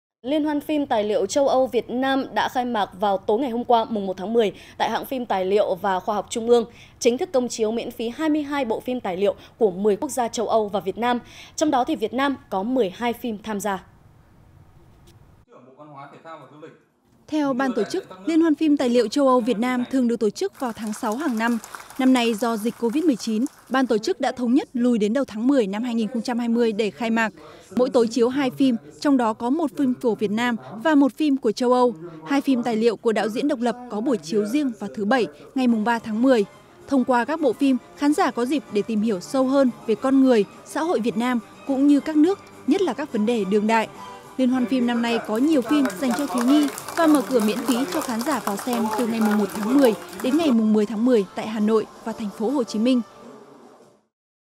Công chiếu 22 phim tài liệu đặc sắc tại LHP tài liệu châu Âu - Việt Nam
Công chiếu 22 phim tài liệu đặc sắc tại LHP tài liệu châu Âu - Việt Nam | Truyền Hình Nhân Dân
1. Dự Báo Thời Tiết Hôm Nay